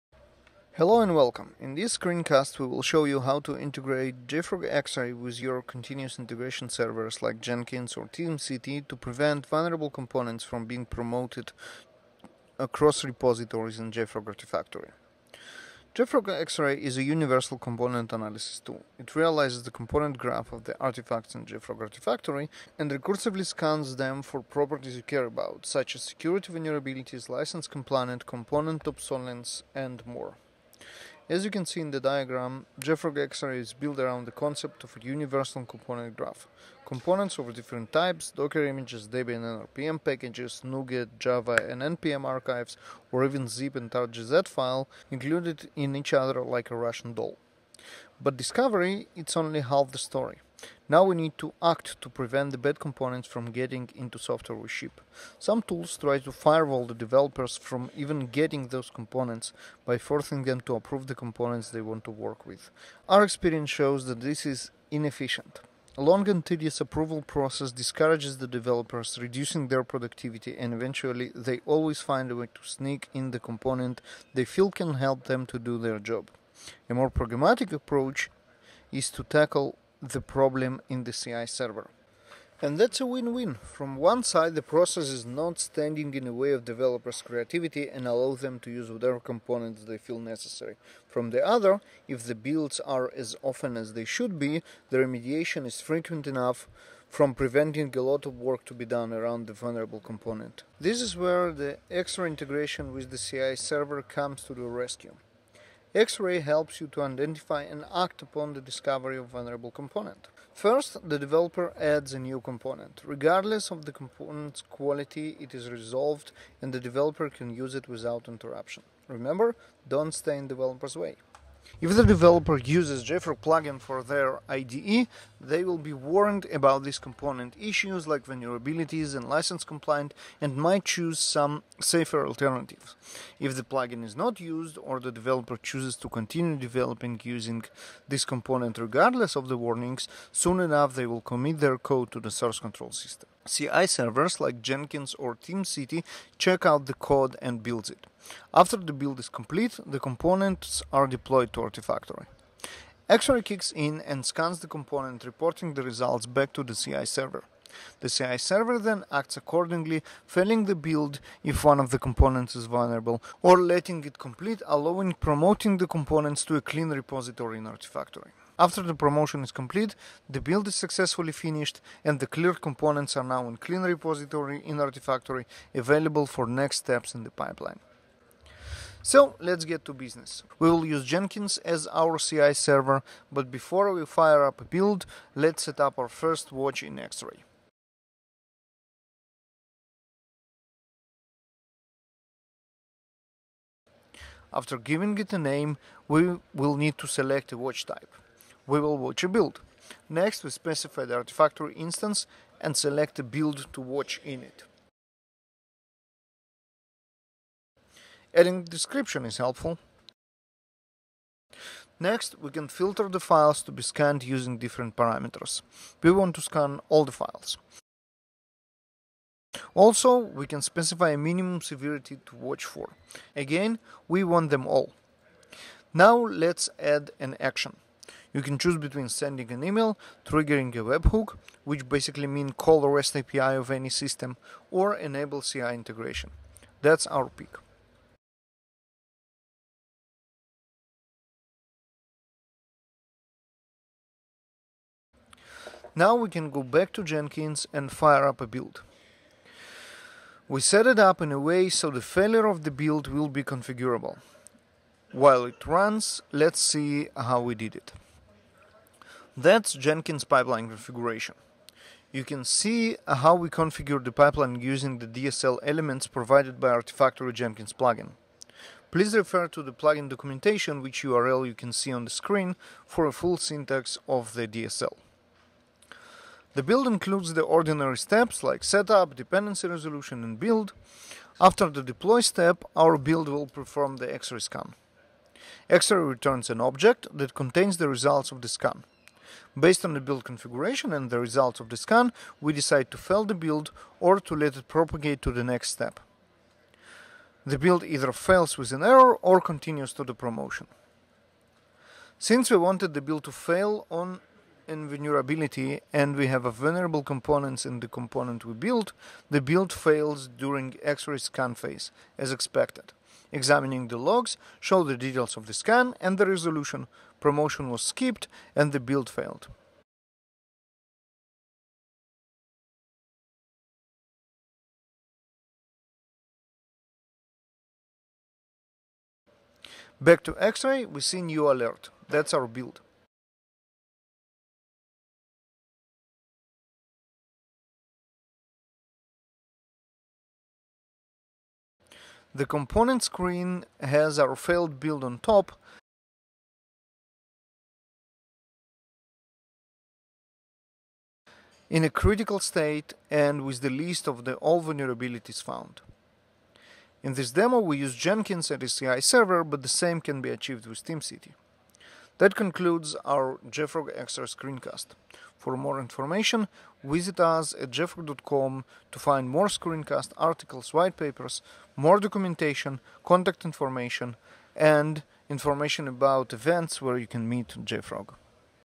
[Screencast] Integrate JFrog Xray with your CI to get peace of mind without getting in devs' way
Get the best of two worlds - developer productivity and safety, by scanning the results of every build for security vulnerabilities, license compliance issues and more with JFrog Xray.